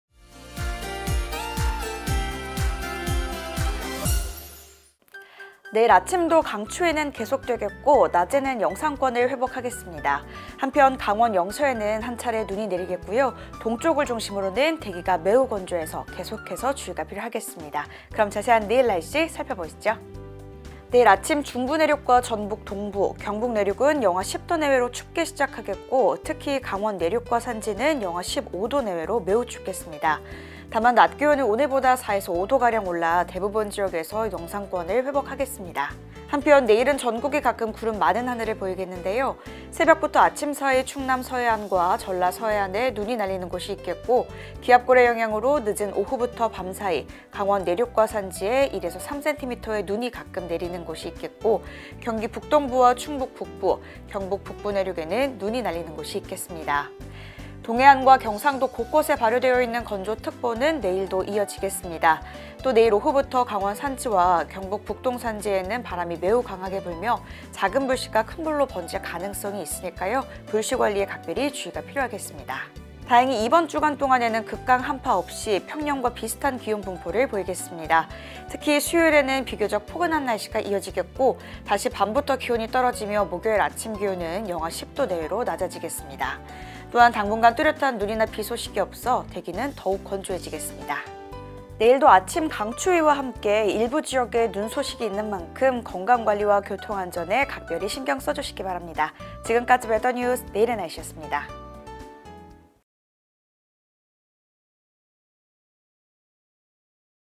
[웨더뉴스] 내일의 날씨 (01월 29일)
내일 아침도 강추위는 계속되겠고 낮에는 영상권을 회복하겠습니다.

강원 영서에는 한차례 눈이 내리겠고요.
동쪽을 중심으로는 대기가 매우 건조해 계속해서 주의가 필요하겠습니다.

오늘의 캐스터 : 김지수

더욱 자세한 날씨가 궁금하다면 웨더뉴스 날씨 정보 보러 가기